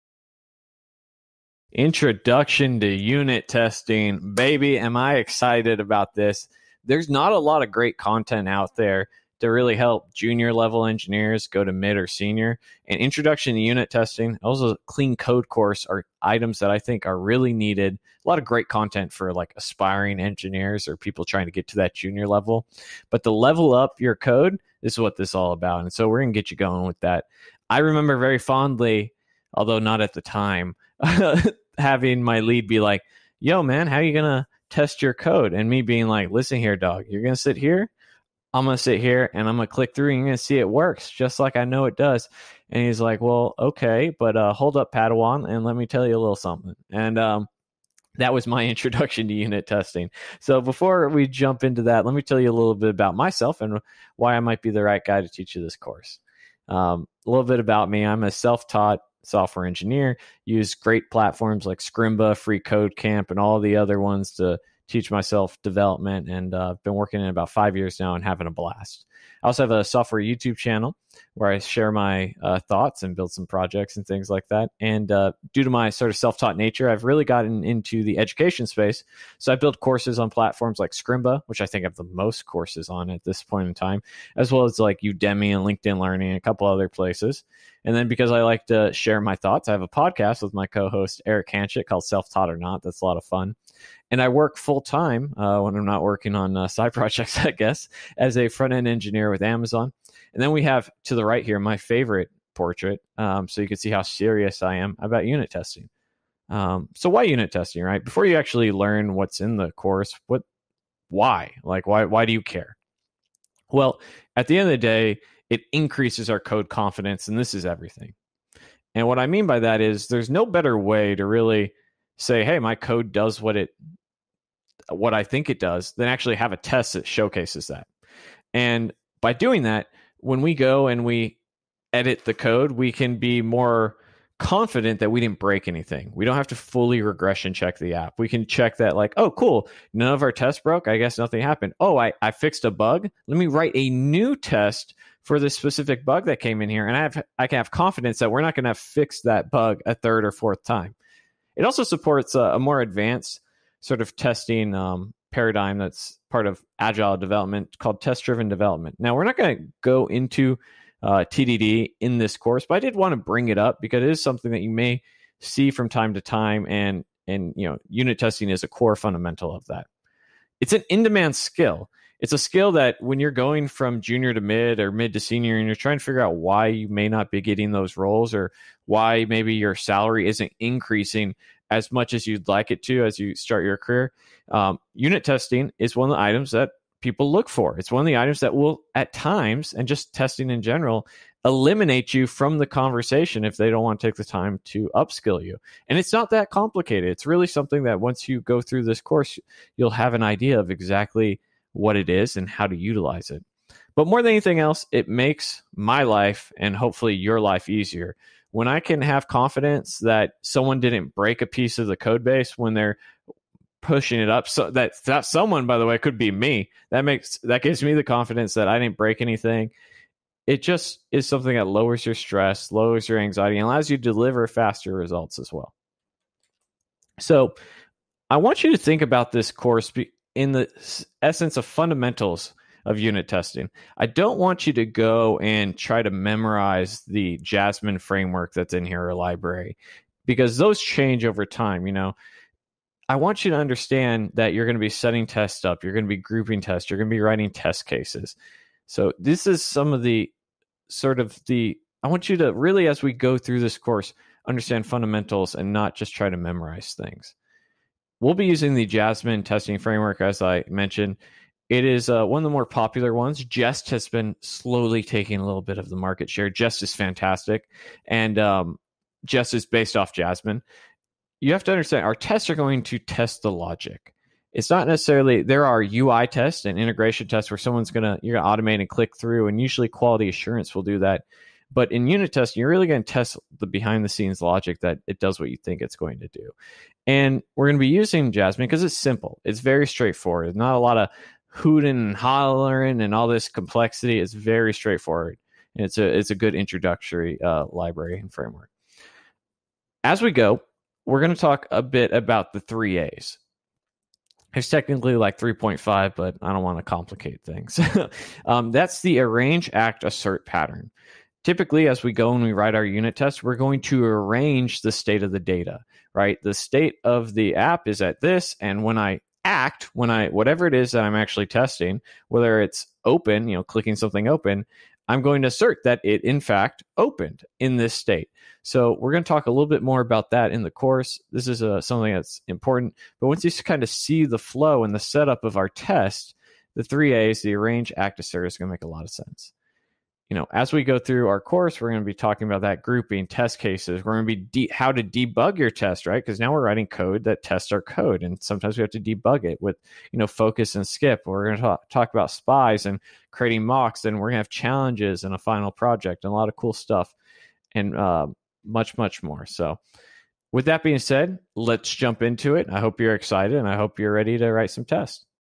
What is unit testing | Unit testing tutorial
Let's dive into what unit testing is and why its important for all developers to learn in this quick, one-hour tutorial.

Code with confidence and lower your stress levels by learning how to test your code as you write it!

Testing your code allows you to check that your logic works as expected every time, giving you code confidence, lowering stress levels, and allowing you to deliver faster results.

This course covers test grouping, test cases, debugging tests with focus, spies and more in just one hour, and contains plenty of challenges to cement your new knowledge.

The earlier you start incorporating unit tests into your code base, the better - but it’s never too late. That means that this course is suitable for both newbie coders and those already working in the field who want to level up.